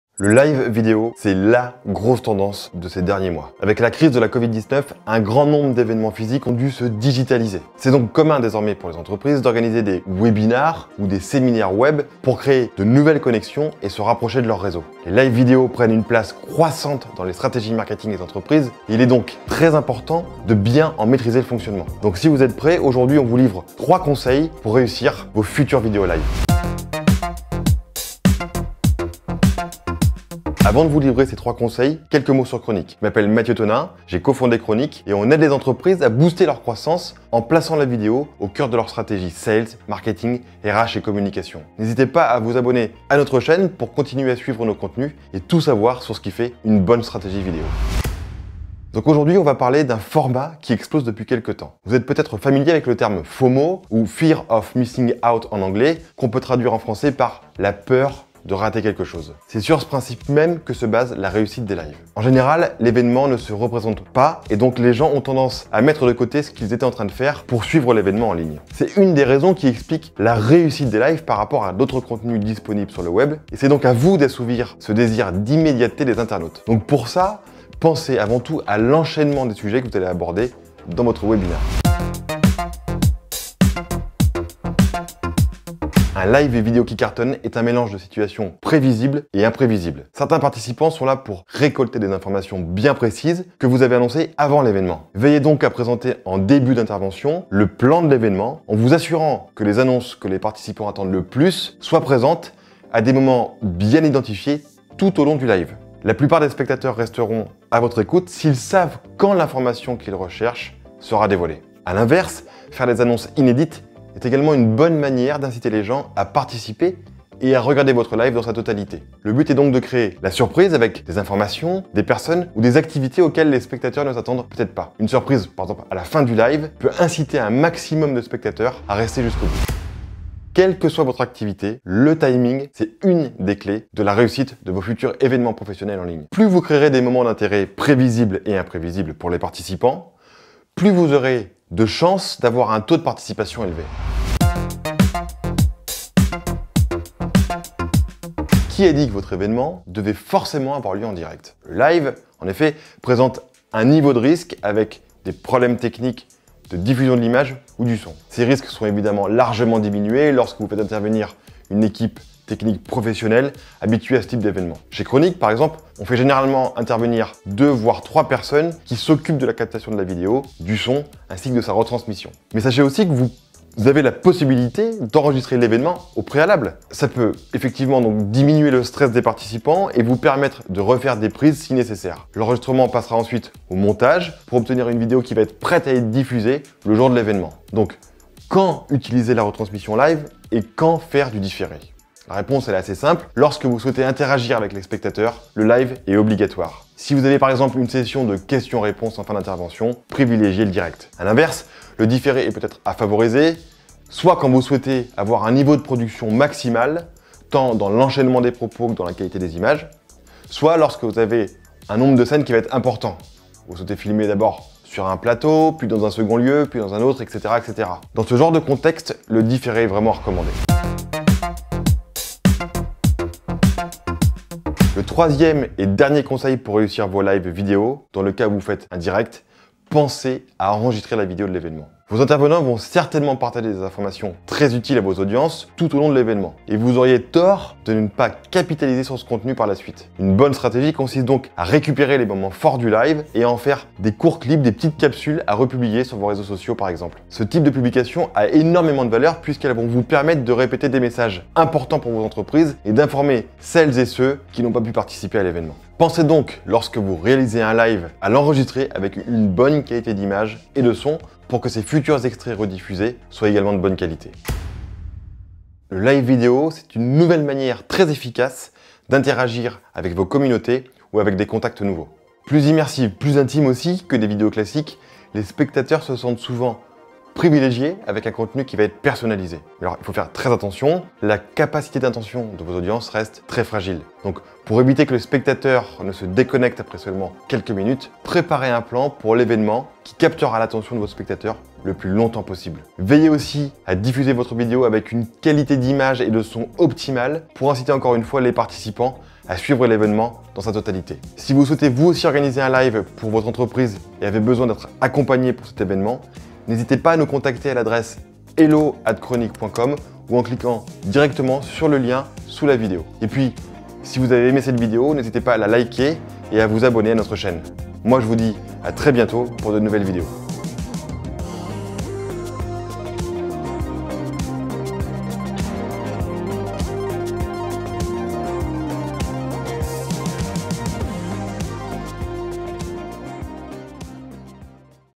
3 conseils pour réussir le live vidéo de son entreprise
Vous souhaitez vous rapprocher de vos audiences, stimuler votre communauté et faire passer vos messages ?

Faites place au LIVE !

Avec la réduction des événements professionnels physiques, les webinars sont devenus les nouvelles armes marketing des entreprises qui souhaitent créer de nouveaux contacts avec des prospects potentiels.
Encore faut-il maîtriser ce nouveau format vidéo et fournir à ses audiences un contenu  qui génère les meilleures opportunités.

Aujourd’hui, Mathieu Tonin CEO de kronik, décortique pour vous les bonnes pratiques du Webinar en vous livrant 3 conseils pour réussir le live vidéo de votre entreprise.

Si vous souhaitez intégrer vous aussi les Webinars dans la stratégie marketing de votre entreprise, contactez-nous ici :